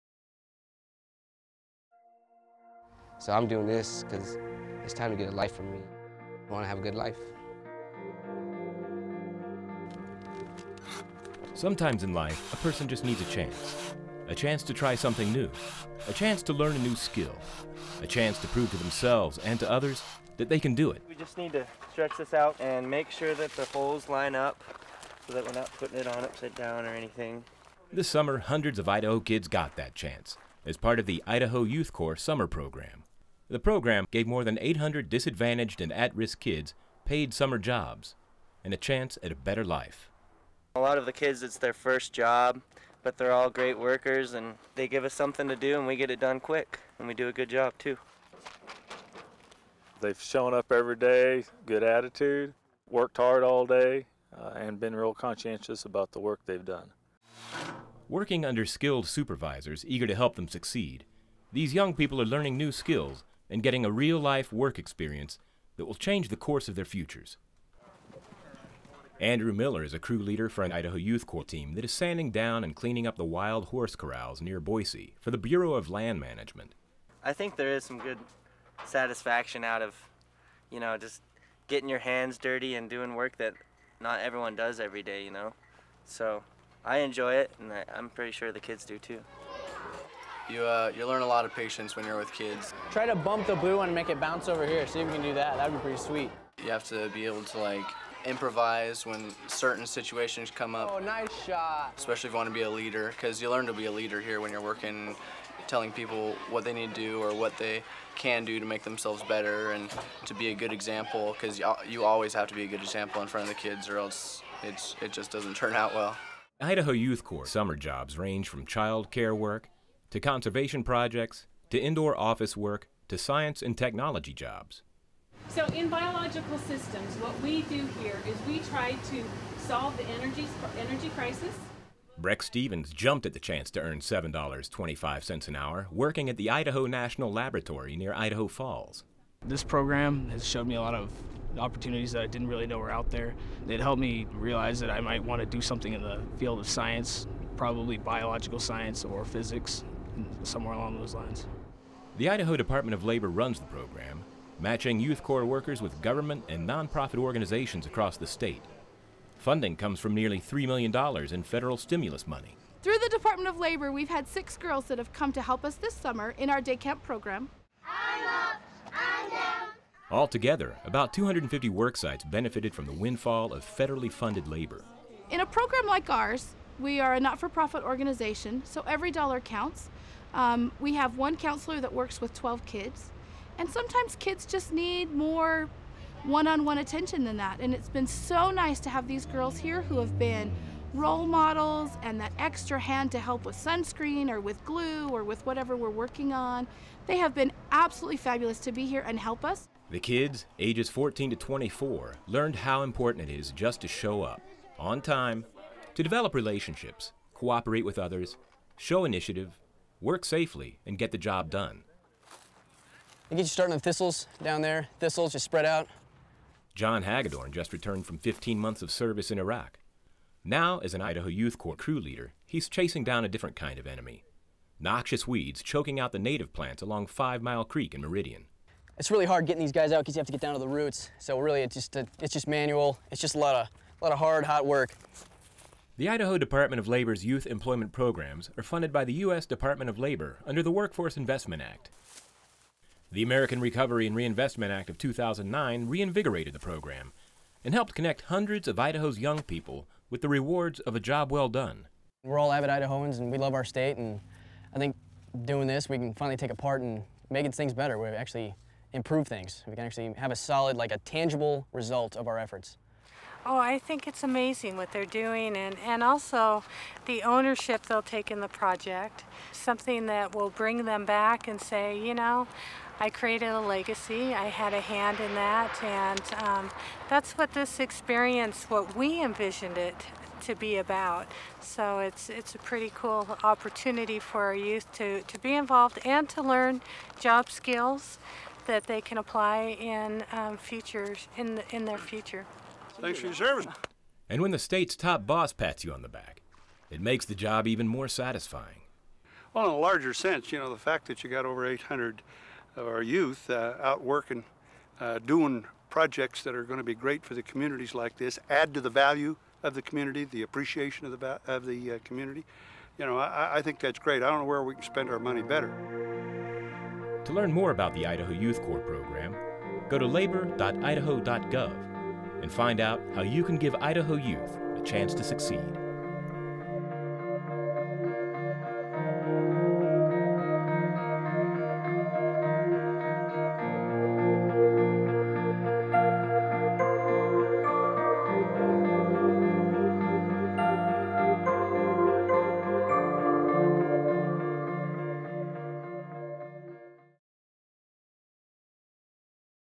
Idaho Youth Corps - 2009
They worked in science labs, museums, libraries, parks, schools and maintenance operations. Hundreds more fanned out across Idaho's forests and range lands, building and maintaining trails, helping crews reduce fuel loads, enhancing wildlife habitat and restoring damaged ecosystems.

In just three short months, a $2.9 million stimulus grant to the Idaho Department of Labor was converted into eight-week summer jobs for 840 young Idahoans between the ages of 14 and 24.

As a result, many of these young people learned new skills and received a real-life work experience.

Idaho Governor C.L. "Butch" Otter has pledged to try and continue the program. And while the federal government provided the money to make this program a reality in 2009, the Idaho Department of Labor is looking for partnerships with nonprofit groups and others that will allow us to continue offering this kind of work experience in summers to come.